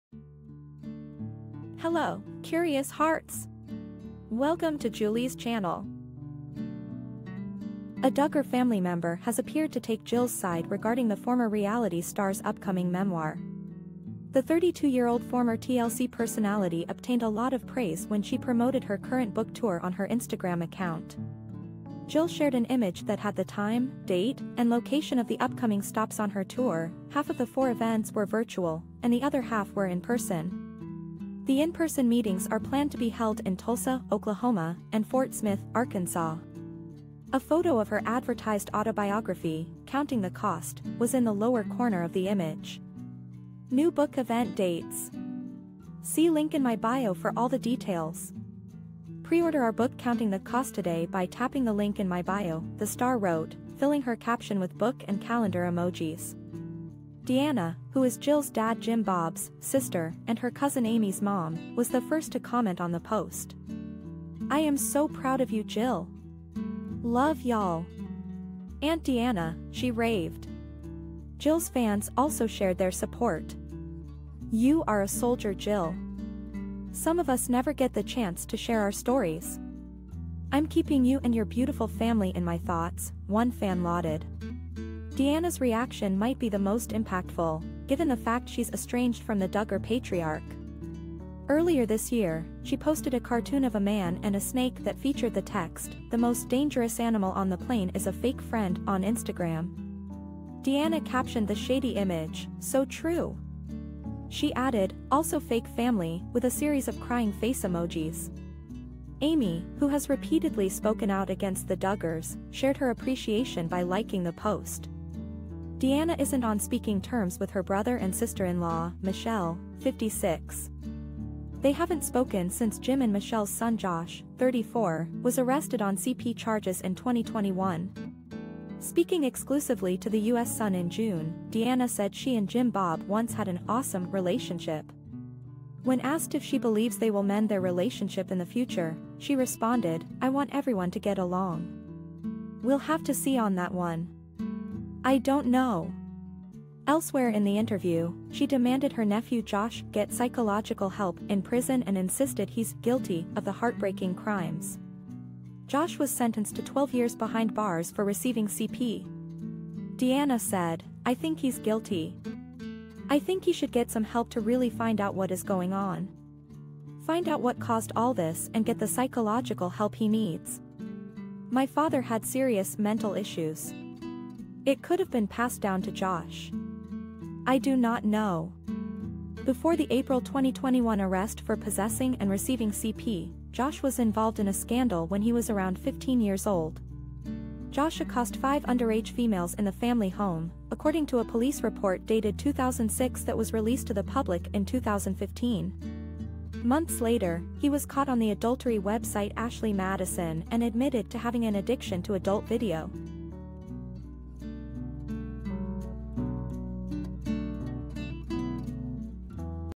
Duggar Member Goes Against Jim Bob As She Praises Jill'S New Tell-All Memoir
A DUGGAR family member has appeared to take Jill's side regarding the former reality star's upcoming memoir.

The 32-year-old former TLC personality obtained a lot of praise when she promoted her current book tour on her Instagram account.

Jill shared an image that had the time, date, and location of the upcoming stops on her tour - half of the four events were virtual, and the other half were in person.

The in-person meetings are planned to be held in Tulsa, Oklahoma, and Fort Smith, Arkan"""